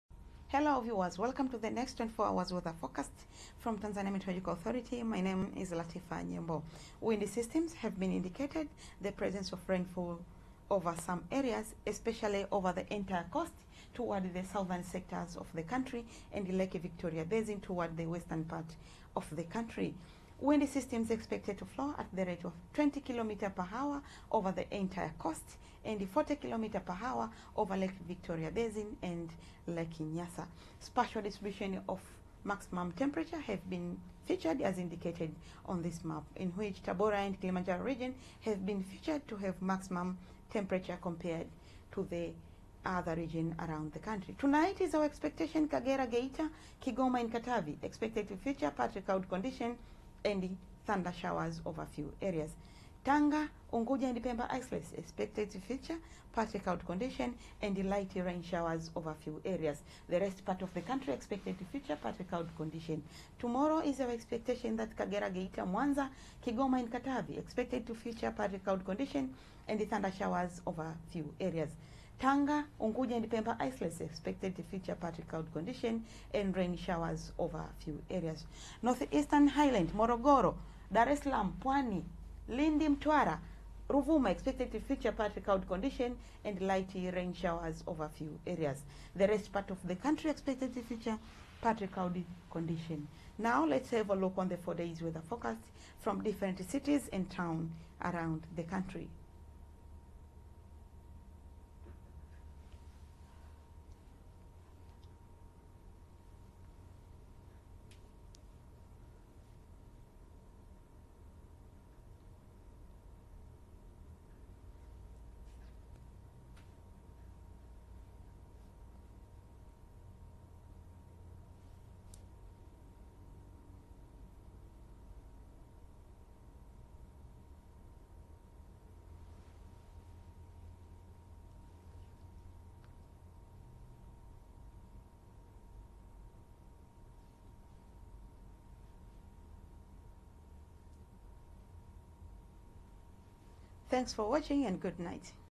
TANZANIA WEATHER FORECAST 26/10/2021
Weather forecast for the next 24 hours starting at 21:00 tonight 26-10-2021 presented by the weather analyst Latifa Nyembo.